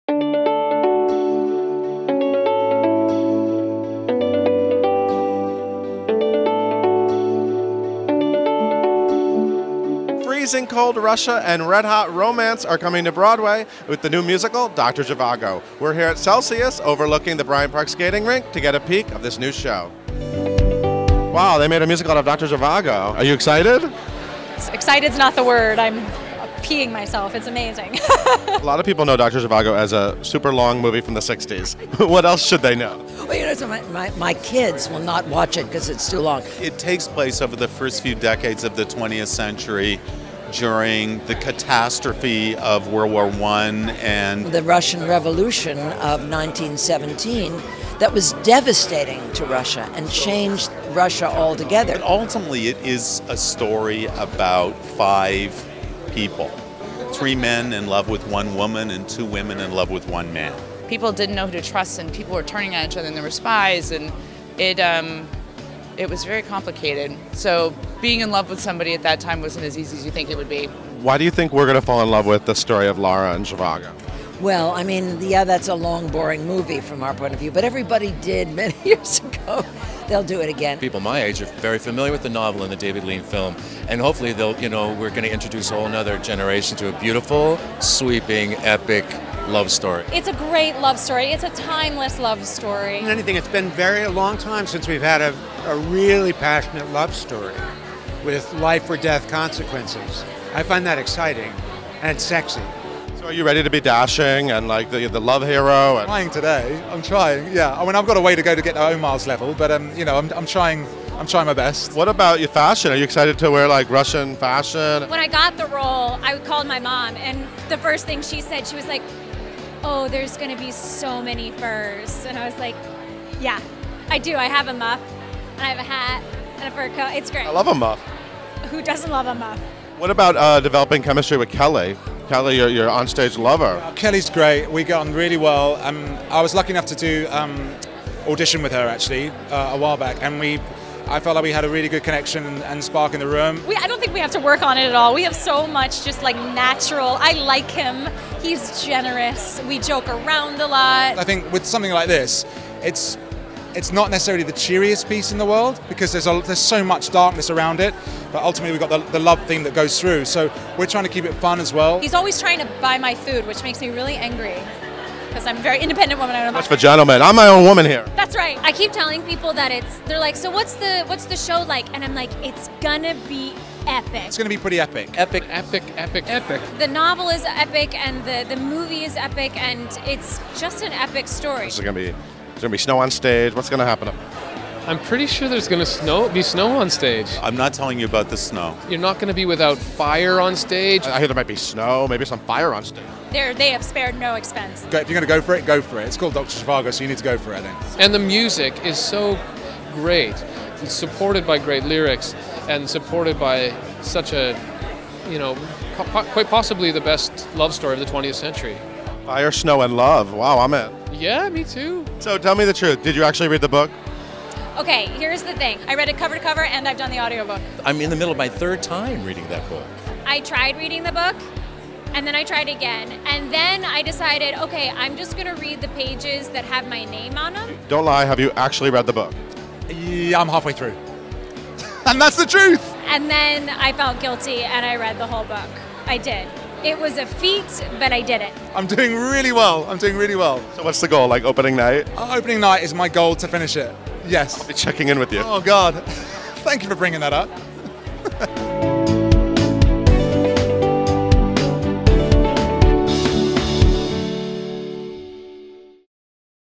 Tam Mutu, Kelli Barrett & More on the History, Sex, Furs, Snow and Fire of Broadway's DOCTOR ZHIVAGO
Paul Wontorek interviews the stars and creators of DOCTOR ZHIVAGO: Tam Mutu, Kelli Barrett, Tom Hewitt, Paul Nolan, Lora Lee Gayer, Lucy Simon, Des McAnuff, Kelly Devine and Michael Weller.